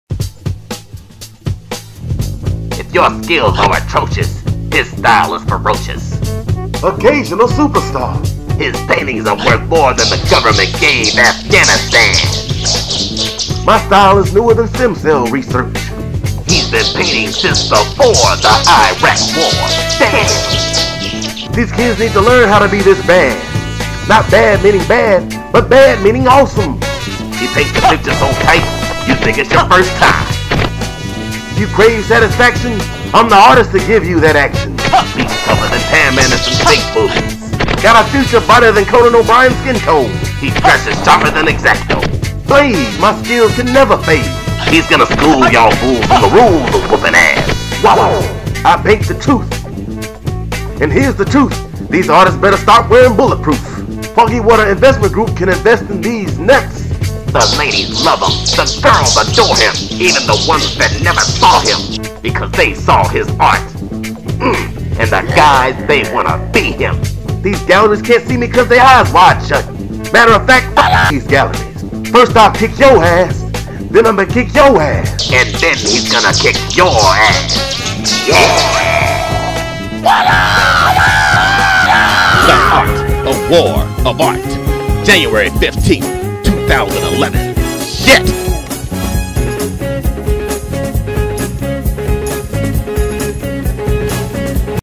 Occasional Superstar Returns: The WWAF Presents The Art Of War Of Art
Stuart McClean Gallery
684 John Wesley Dobbs Ave, Suite A,

The 4th installment in The World Wide Art Federation Art Battles, The Art Of War Of Art is sure to blow your mind. The most exciting art exhibition in the South and we're betting the east coast too, is happening on Jan 15, 2011. Starring some of the art world's finest artist from the contemporary to traditional fine art realm.

Starring:

Fabian Williams as Occasional Superstar

Fahamu Pecou as Sex Choc

Charly Palmer as Montrell The Merciless

Maurice Evans as Evolution

Jessica Scott Felder as Lady Luxe

CP The Artist as Apocalypse

Grace Kisa as Strange

Tela Hubbard as Queen T

Jasiatic Anderson as Camera Lensey

Abu as Kung Fu Abu

and introducing

Aja Adams

Fabian Williams aka Occasional Superstar and Fahamu Pecou aka Sex Choc, put their blood feud aside to take on a foe that threatens to annihilate creativity in both WWAF artists and mere mortals. A seedy, dark, privately, private held hedge fund known as the Foggy Water Investment Group (The Man or FWIG), has been seeding their influence in the halls of Congress as well as the WWAF. By taking art out of schools, they hope to cultivate a worker bee society who only does as they're told and obeys all authority -- even to their own detriment.

The FWIG has infiltrated the WWAF through social connections to push for budget reductions in the art battles. They've gone as far as dispatching agents of distraction during the battles to disrupt the creativity. They even have artists lobbying for their interests. The brainwashed artists, sadly, have adopted some of the FWIG view points: "You don't need art education. You can paint in your spare time. Hell, just do arts and crafts at home."

The FWIG's singular goal is to eliminate artistic expression everywhere, thus making the public at large ripe for conformity. They want to rule the world and only a ragtag band of rebel artists can stop them. Together they will use The Art Of War Of Art to defeat this powerful menace and all their allies!